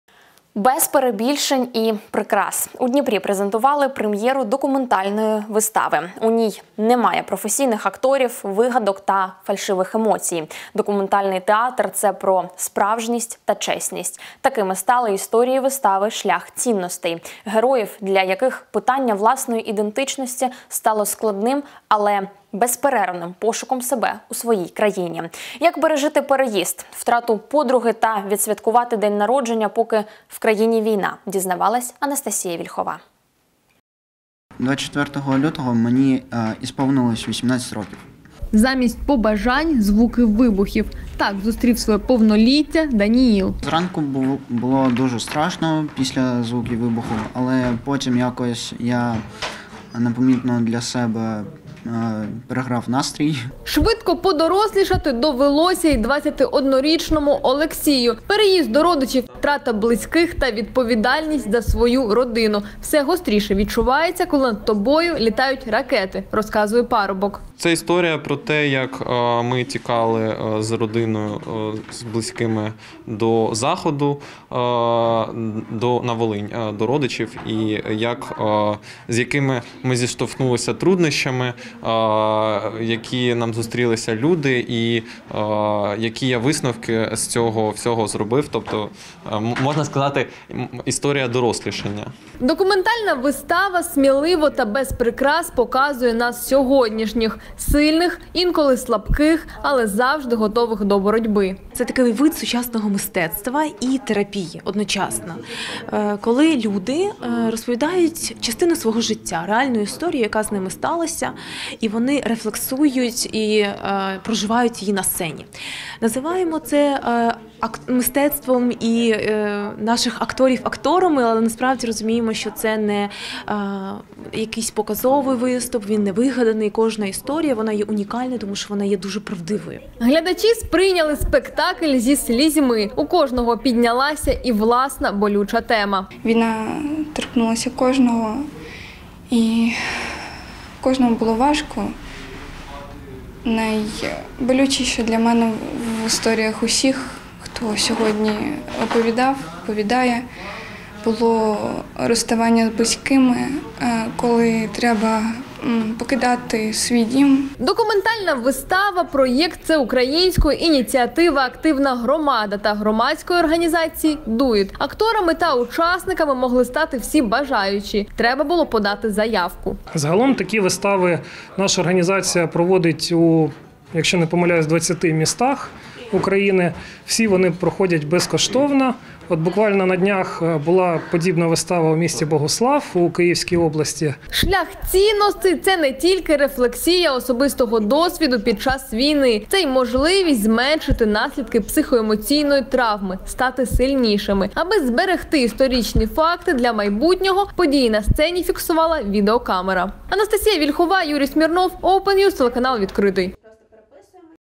«Шлях цінностей»: у Дніпрі презентували документальний театр
Без перебільшень і прикрас. У Дніпрі презентували прем‘єру документальної вистави. У ній немає професійних акторів, вигадок та фальшивих емоцій. Документальний театр – це про справжність та чесність. Такими стали історії вистави “Шлях цінностей” – героїв, для яких питання власної ідентичності стало складним та непростим, але безперервним пошуком себе у своїй країні. Як пережити переїзд, втрату подруги та відсвяткувати День народження, поки в країні війна? Дізнавалася Анастасія Вільхова